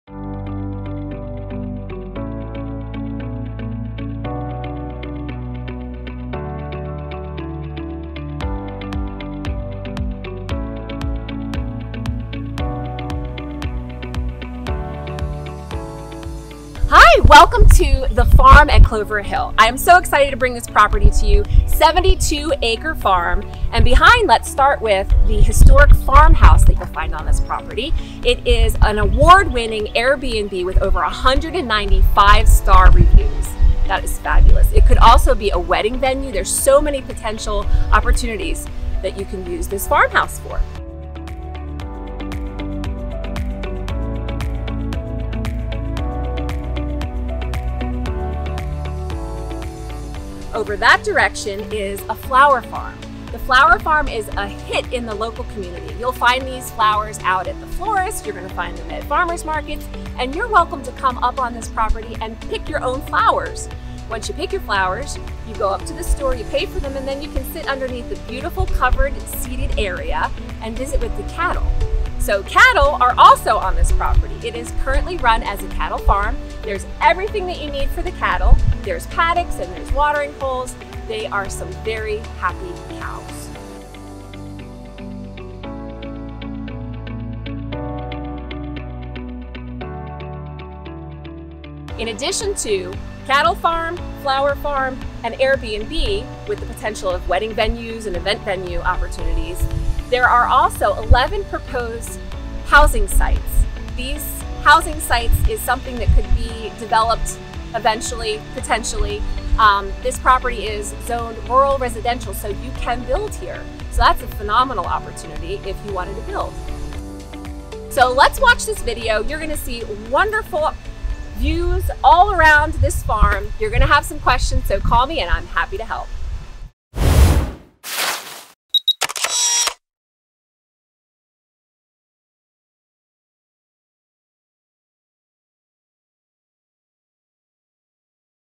Aerial Tour - The Farm at Clover Hill with Jen Avery, REALTOR
The Farm at Clover Hill - 4022 Rockland Rd, Front Royal VA
4 BEDS | 2.5 BATHS | 72.59 ACRES | 4,633 SQ FT | 1781 YEAR BUILT
Welcome to The Farm at Clover Hill! 72 Acre Farm | Historic Farmhouse Circa 1781 | Successful Airbnb | Flower Business Building & Crops | Ice House | Corral | Barn | General Development Plan for subdivision (11 house sites.)
Unique opportunity to purchase land that does not have a conservation easement in the Rockland area! Have you dreamed of owning a farm and living in a historic farmhouse in Virginia? This could be the perfect property for you! OR would you like to build your dream house with gorgeous views of the Blue Ridge Mountains, while running a successful Airbnb and flower farm? Or would you like to own enough land to build an additional home or two for family? How about running a breathtaking wedding venue? There are endless possibilities at The Farm at Clover Hill!!
Currently cattle are run on the farm. There is a barn, corral, feeders, hand dug well that waters the cattle, and 8 bay shed. There is fencing in place to keep cattle safe. The gorgeous historic farmhouse has been renovated and updated. It has been running as an Airbnb for years (the 6th Airbnb approved in Warren County!) The new owners can chose to live in the farmhouse or continue the business! Behind the house is the "ice house." This building was built to resemble the original ice house. It can be used for events as a gathering space, perfect for cocktails at sunset! There is a large 19 x 13 deck to enjoy. Sellers had a vision to expand into a wedding venue. Currently, there has been 2 conditional use permits approved for this property. 1. Events, to include flower shop. 2. Short term vacation rental. New owners would need to reapply. FARMHOUSE: Enjoy a cheerful enclosed front porch as you first enter! MAIN LEVEL: The main level has been updated, painted, and restored. There is a large parlor/den perfect for relaxing and playing a round of cards in the evening. Also on the main level is comfortable library/living room with books the owners found in the attic! There are numerous sets of step to the upper level throughout, total of 3. The kitchen, formal dining room, and utility room/laundry room will not disappoint. The kitchen has stainless steel appliances, island, sitting area with gas fireplace, and modern conveniences. The main floor also has a full bath and half bath. UPPER LEVEL: Four spacious bedrooms are on the upper level. Multiple sitting areas create a perfect space for potential wedding preparations and parties.
BUILDING OPTION: There has been general development plan designed for 11 house sites on this property. See plat in view docs. Property is zoned rural residential.
FLOWER BUSINESS: The building used as the flower shop is 19x13 inside with a covered porch (with ceiling fans) 19x13. There are fields of herbs and flowers with electric fence for protection.
GOLF COURSE ACCESS: Property is close to three top golf courses in town. Within one mile enjoy Bowling Green Country Club, Shenandoah Golf Club, and Bowling Green South Golf Course.
CLOVER HILL HISTORY: 1781 Two story Log Home on Clover Hill mentioned in the deed. 1820 Federal Style (Front brick section) added. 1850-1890 Queen Ann section added. See view docs for more details.
Jennifer Avery, REALTOR® “Your Happy Home Expert!”
BPOR, SRS, CNE, E-Pro Certified | Licensed in VA